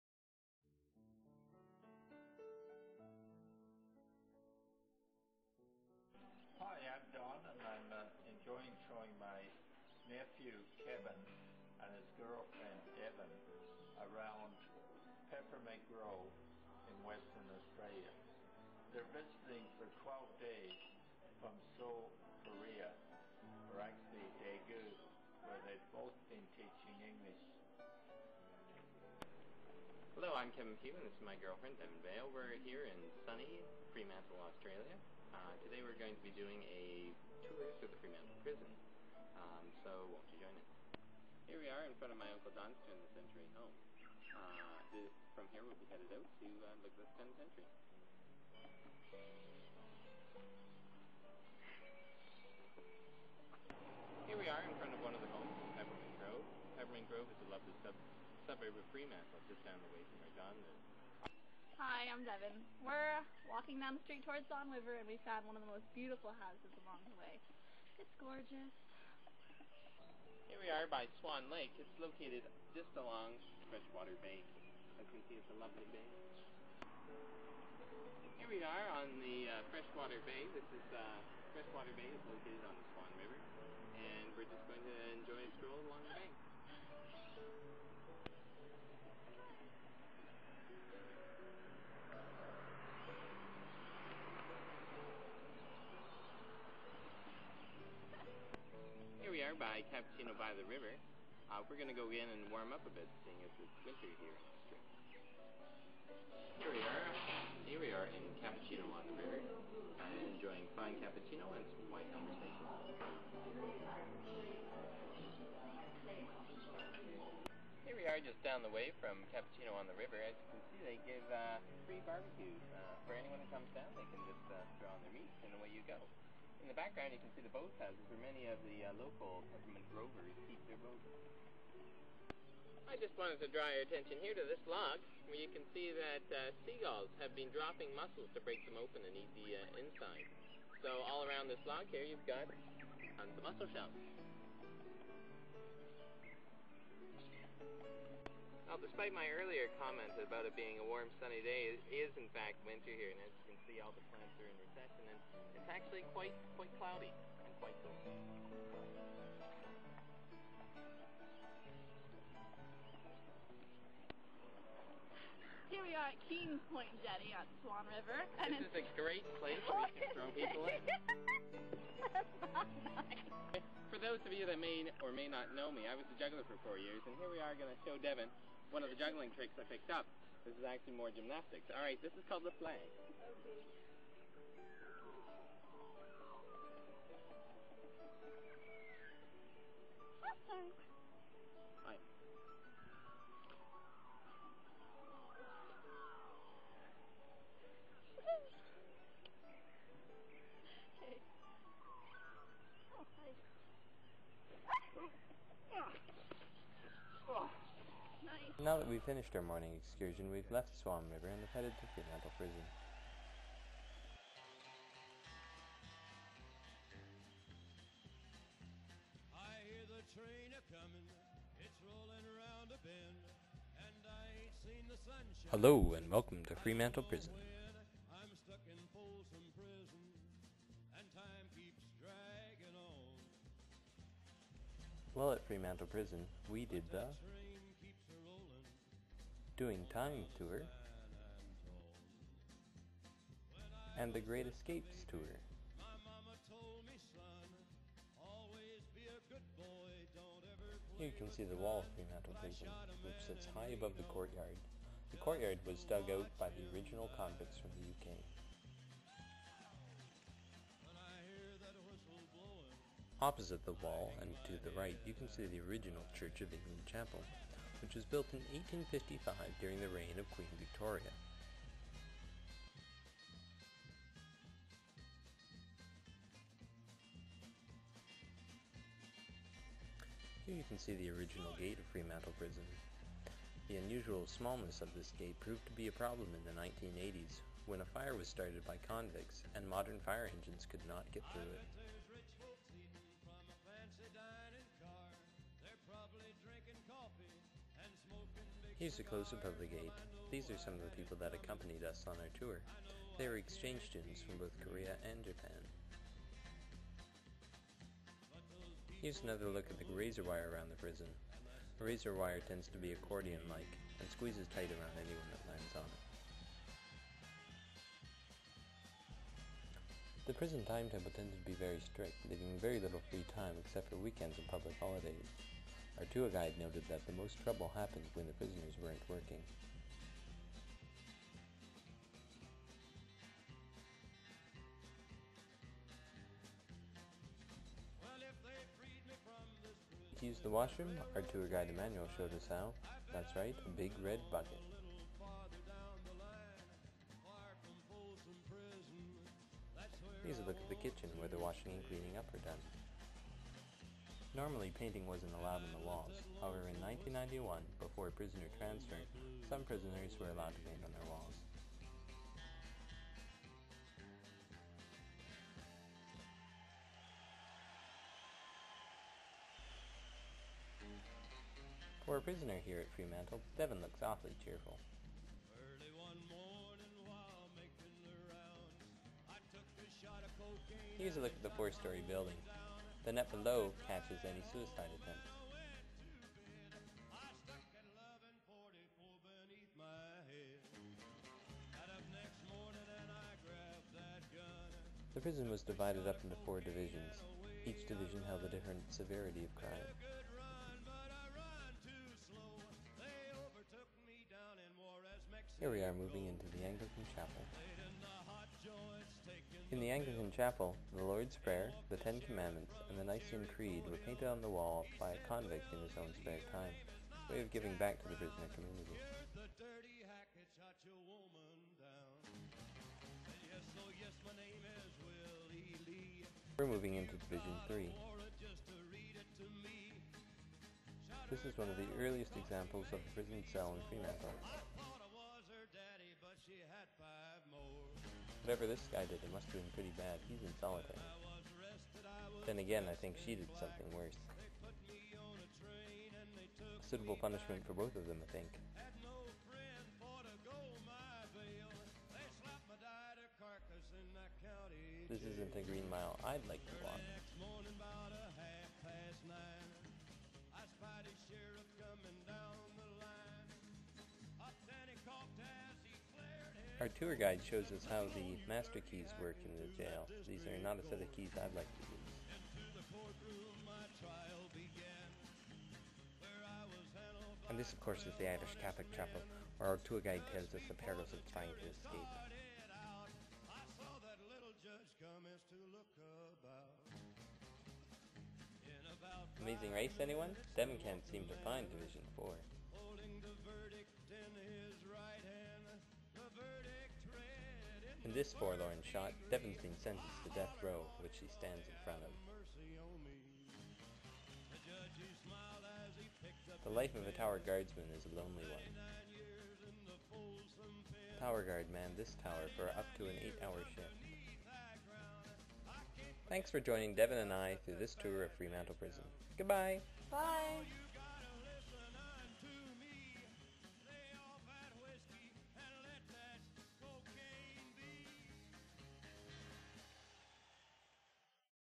Fremantle Gaol / Prison WA WESTERN AUSTRALIA, PERTH
Fremantle Prison AA 2006
We completed our visit to the historic port city of Fremantle with a guided tour of Fremantle Prison, the largest and most intact convict-built cell range in Australia. Originally known as 'The Convict Establishment', the Prison was built by convicts in the 1850's from limestone quarried on the site. It became a colonial prison in the 1880's and its inmates extended to include women from the 1890's through to 1970 and military prisoners and enemy aliens during World War II. Fremantle Prison was decommissioned as the state's primary maximum-security goal in November 1991 and the precinct is now recognised nationally and internationally as one of Western Australia's premier heritage sites.
KEVIN PUGH AND DEVON VAIL

DON PUGH